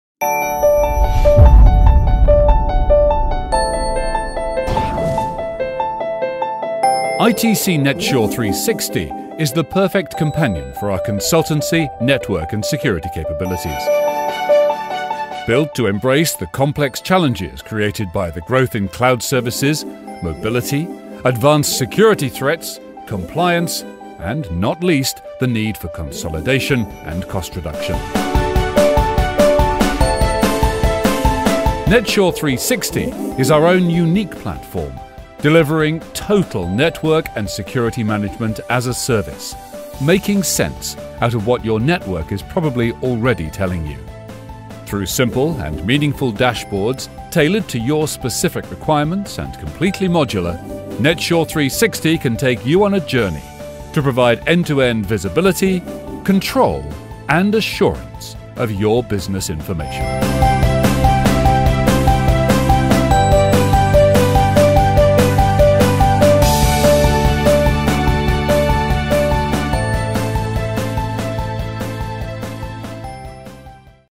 ITC NetSure360° Video
ITC demonstrate the components of NetSure360°